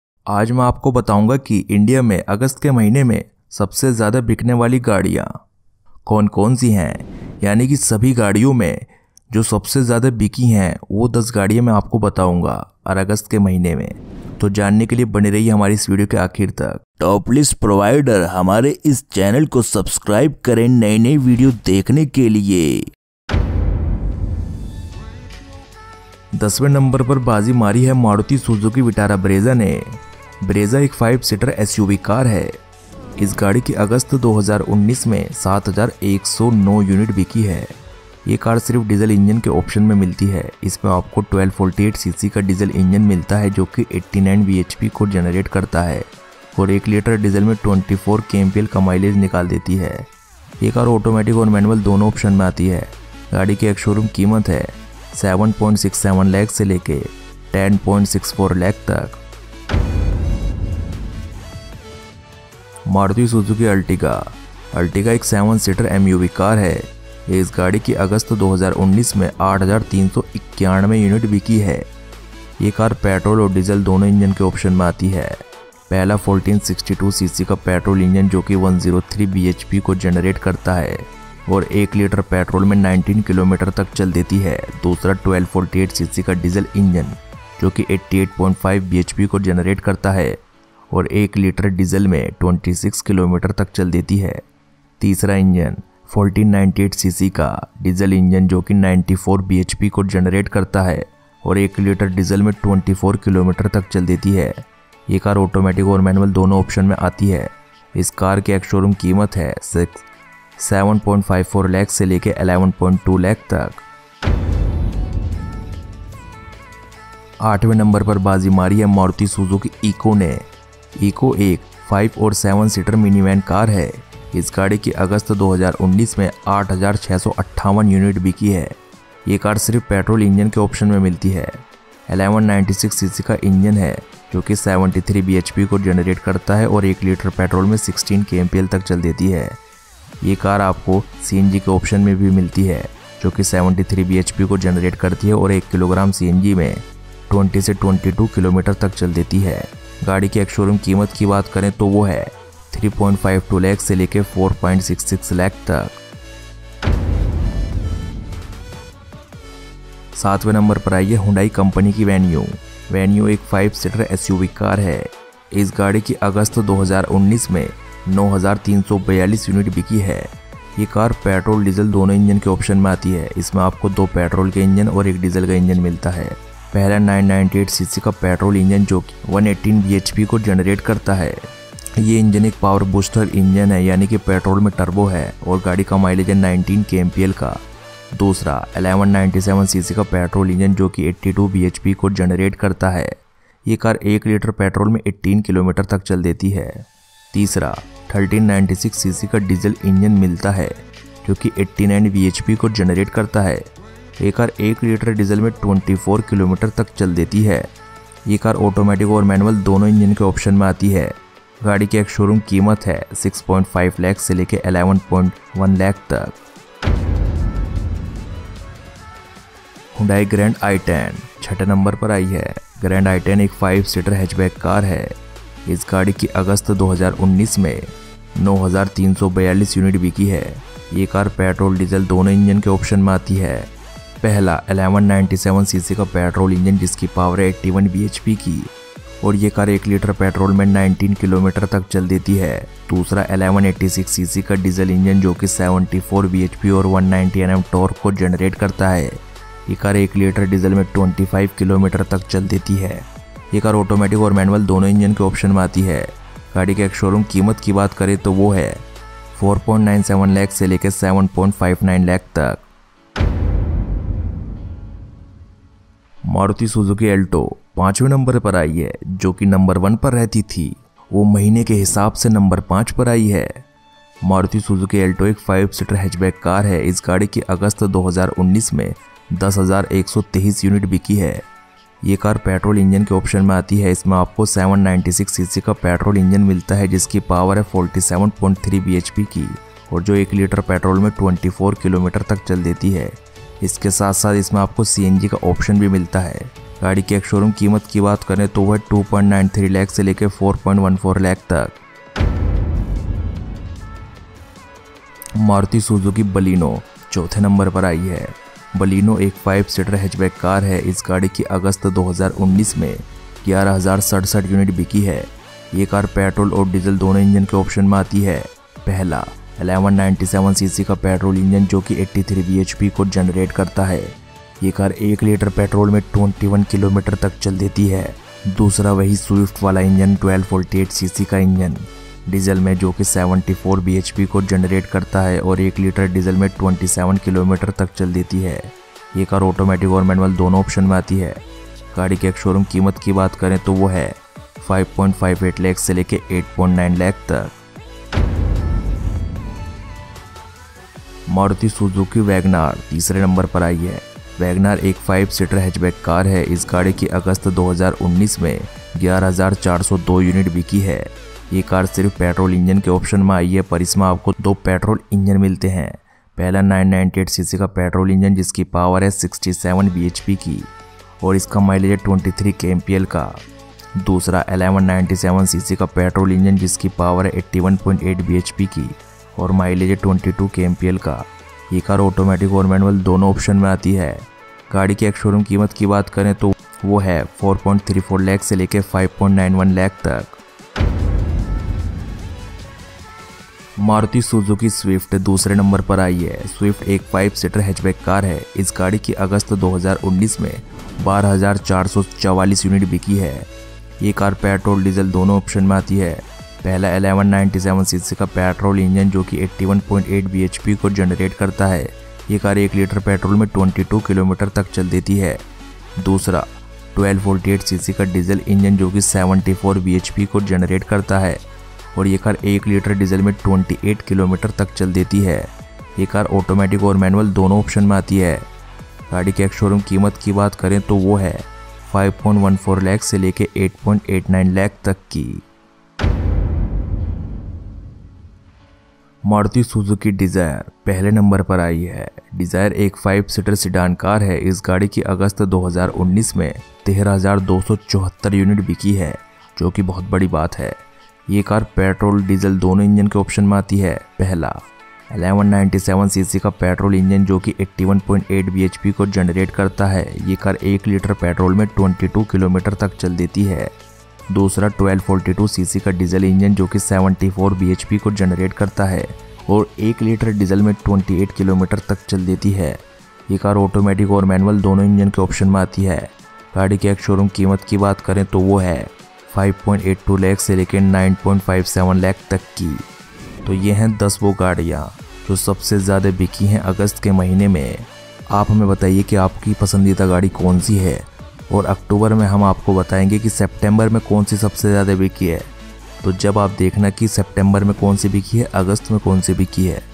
Top 10 Most Sold Cars In August 2019 (In Hindi)
Highest Selling Cars In August
India Best Highest Selling Cars 2019
Most Sold Cars In August 2019

Vitara Brezza - 0:00:23
Ertiga  - 0:01:00
Eeco  - 0:01:56
Venue  - 0:02:37
Grand i10  - 0:03:38
Alto  - 0:04:23
Baleno - 0:05:04
Wagon R  - 0:05:49
Swift  - 0:06:33
Dzire  - 0:07:17